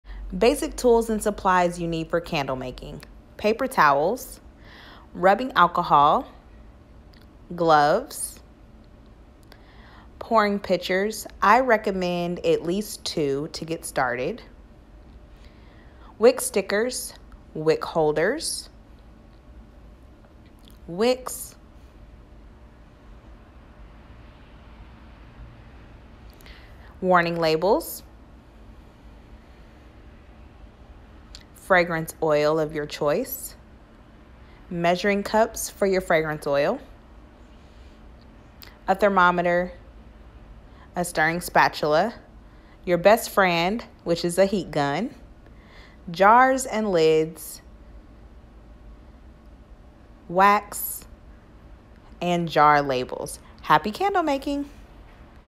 Basic Tools And Supplies For Candle Making
Hey candle makers!!! I see you😩 DO NOT feel you need to buy expensive tools and supplies for your candle making journey!

I see use all of these supplies to this day! I did upgrade to a presto for mass production, but I still use everything mentioned in this clip including printing my labels at home.

Shop at your local dollar store and Amazon. And as
you perfect the craft and coins come, then slowly invest in upgrades! In the beginning, coins are limited and I encourage you to budget accordingly to build your biz🕯🤎

Happy candle making!